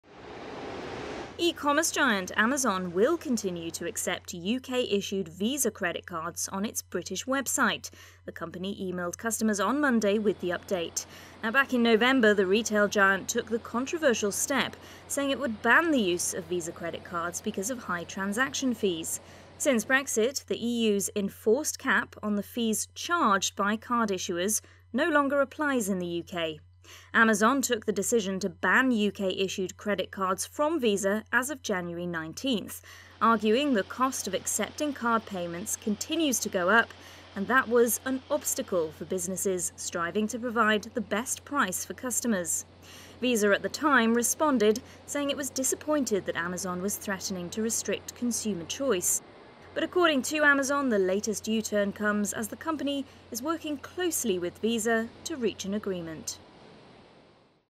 Amazon halts ban on UK-issued visa credit cards
Amazon halts ban on UK-issued Visa credit cards, adding that it was working with Visa to resolve a dispute over payment fees